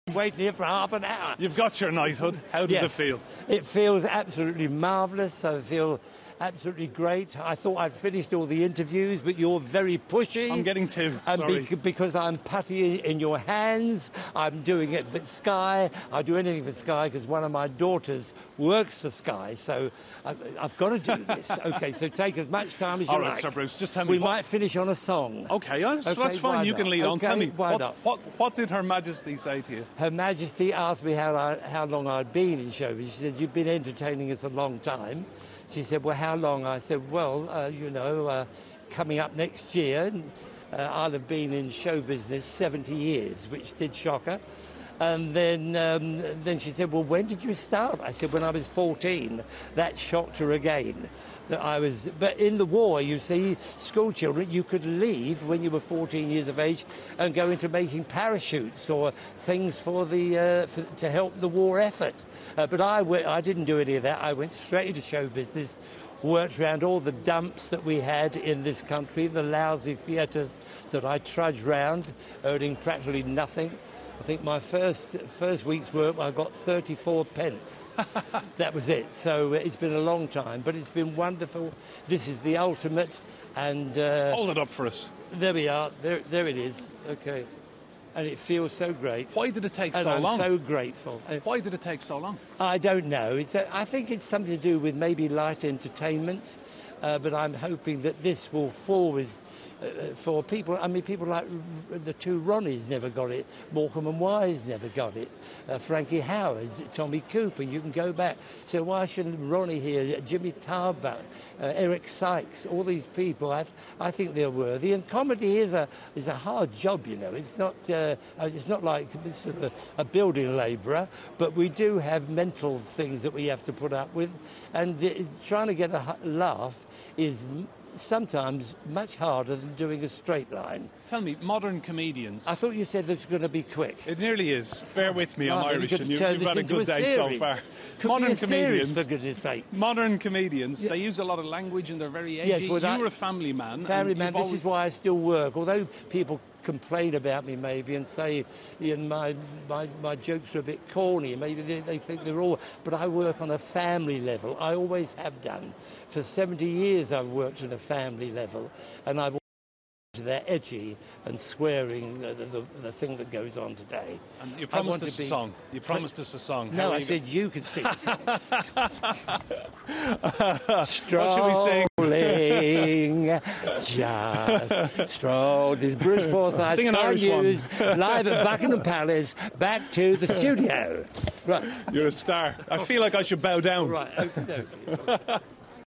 Bruce Forsyth Knighted By The Queen
Sir Bruce Forsyth has received his knighthood for services to entertainment in a career spanning 70 years.
The Strictly Come Dancing host's place as a leading TV presenter has been recognised after years of campaigning by fans.
The award also marks his charity work, something the star does privately.